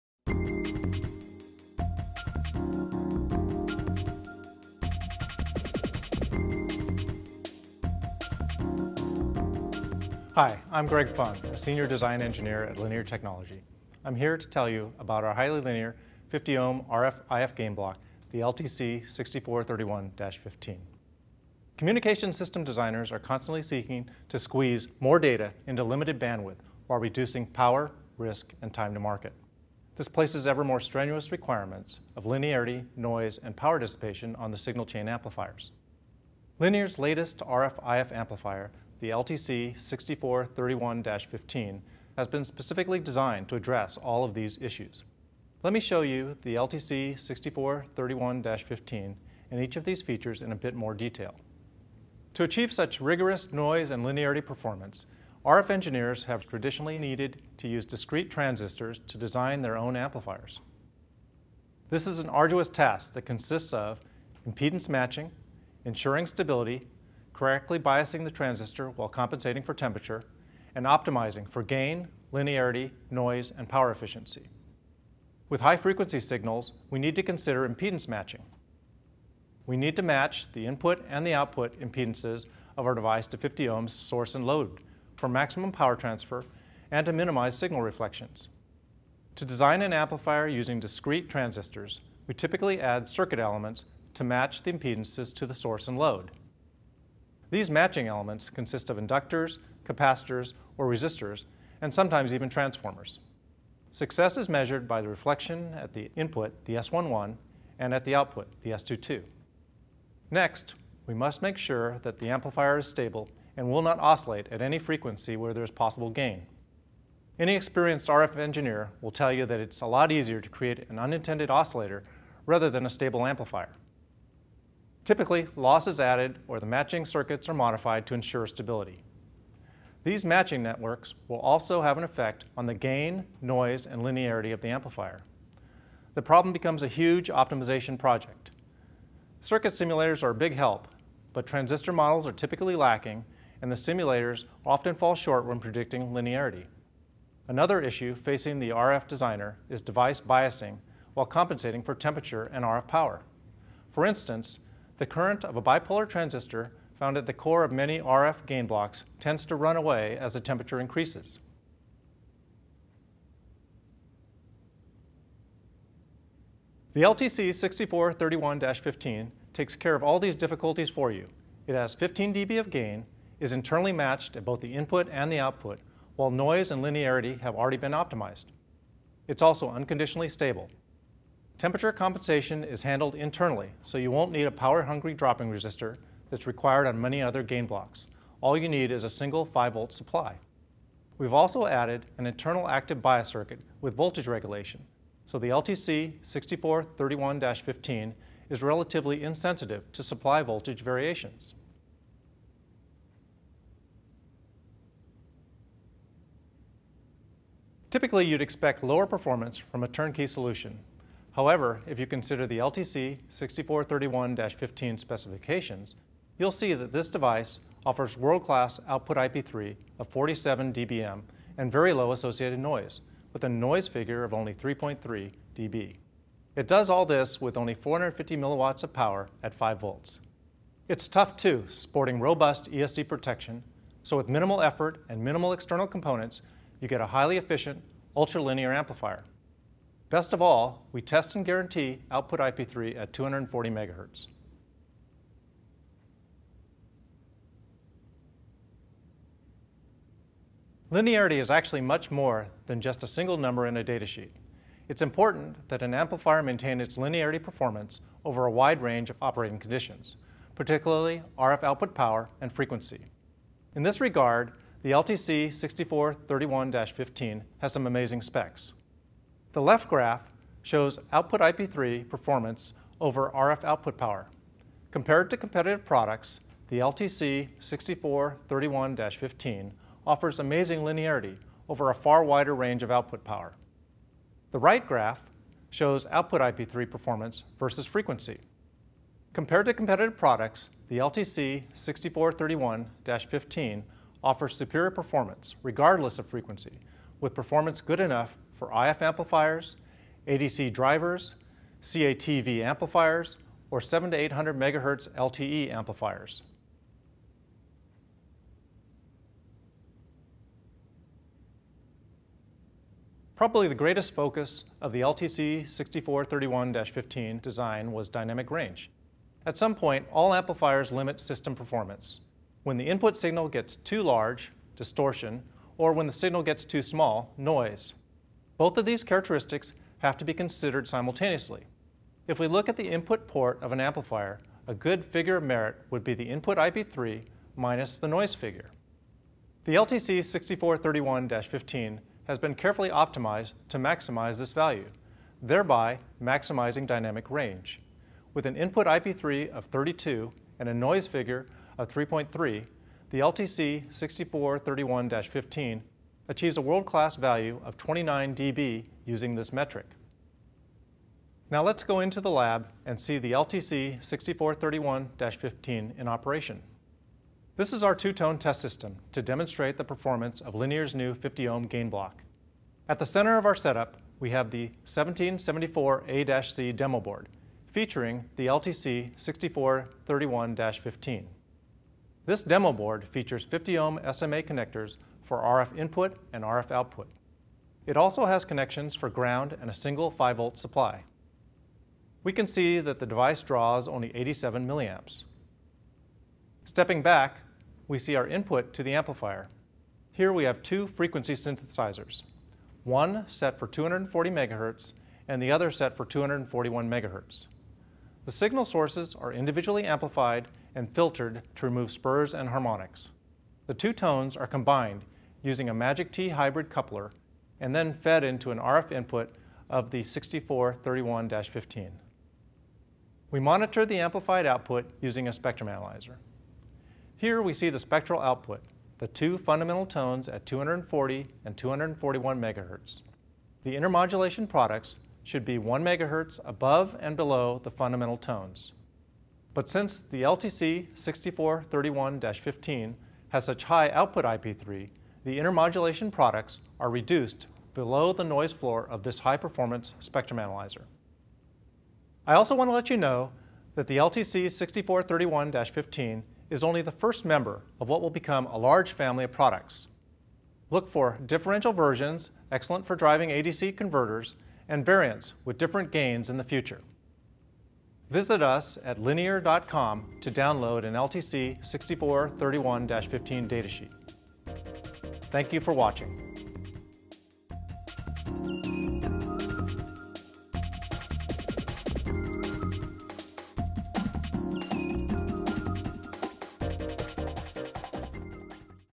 IF/RF Gain Block State-of-the-Art Performance the Easy Way
with Greg Fung, Senior Design Engineer, Signal Conditioning Products

Bandwidth has become more precious than gold.  As we push more and more data wirelessly through our smartphones, tablets, GPS, and video players, the spectrum has become more crowded.  Increasingly complex and efficient modulation schemes allow us to carry more MBits/mHz through the air.  However, the key to these demanding schemes is a high fidelity signal path.  Achieving high linearity and low noise while dissipating a minimum of power are keys to achieving these increased data rates within a limited spectrum and power budget. Similarly, components with built-in ease-of-use and guaranteed specifications are keys to quick, painless design cycles.

Linear Technology's LTC®6431-15 IF/RF amplifier has been specifically designed with all of these attributes in mind. It simultaneously offers both high linearity and low noise while keeping power dissipation in check. At the same time, it offers unmatched ease-of-use and guaranteed minimum linearity specs.

The LTC6431-15 is a 15dB gain amplifier that is internally matched to 50Ω at both the input and output from 20MHz to 1700MHz.  All of the common implementation difficulties of IF/RF amplifiers have been taken care of within the device itself. The LTC6431-15 is unconditionally stable, its internal active bias circuit compensates for temperature variations, and an internal voltage regulator minimizes sensitivity to supply voltage variations.

While easy to use, the LTC6431-15 is no slacker in performance.  It boasts an OIP3 of 47dBm and a noise figure NF of 3.3dB at 240 MHz, making it a perfect choice for many signal chain applications.  Furthermore, it achieves this wide dynamic range while only using 90mA from a single 5V supply.

At Linear, we have gone a step further.  All Grade A LTC6431-15 devices are OIP3 tested and guaranteed for OIP3 performance. With its ability to linearly drive 50Ω, the LTC6431-15 is an ideal amplifier to restore IF filter insertion losses or add gain where needed.  Its linearity is sufficient to drive many 14-and 16-bit ADCs.  And because of its wide bandwidth, this device is an ideal CATV driver, 700MHz-800MHz LTE amplifier or military/satellite communication amplifier.